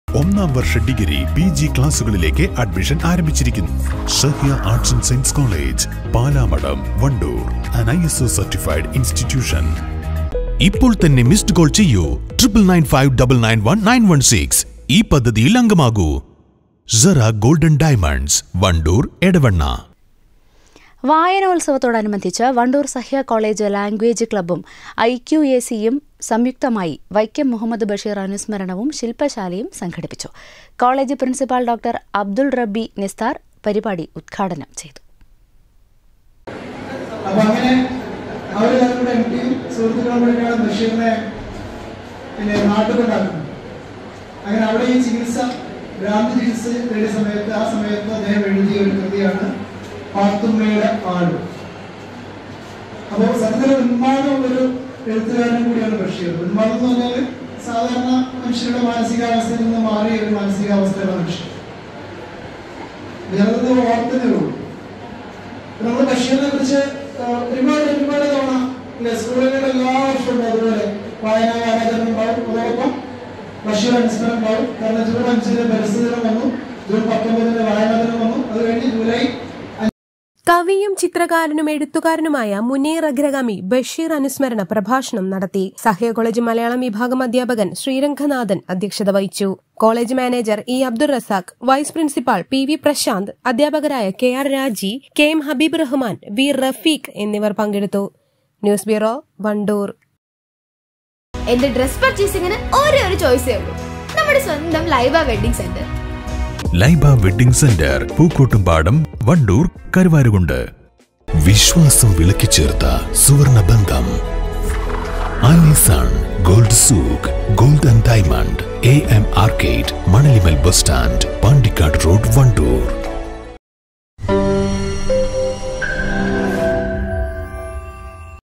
വൈക്കം മുഹമ്മദ് ബഷീർ അനുസ്മരണവും ശില്പശാലയും
പ്രിൻസിപ്പാൾ ഡോ. അബ്ദുൽ റബ്ബി നിസ്താർ  പരിപാടി ഉദ്ഘാടനം ചെയ്തു.